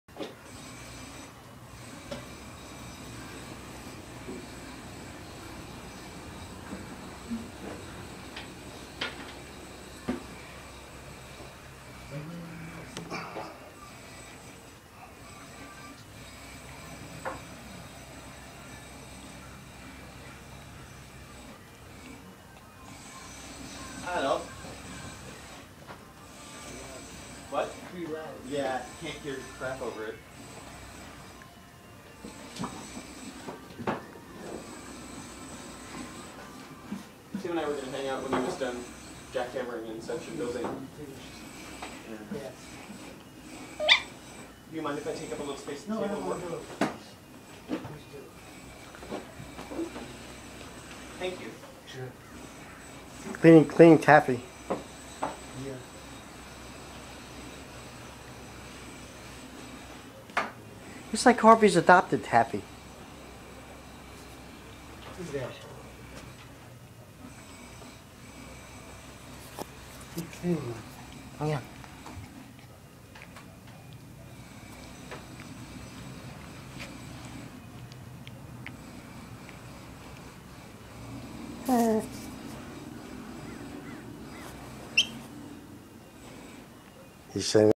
cat licks puppy clean
Harvey the cat loves little Taffy the dog.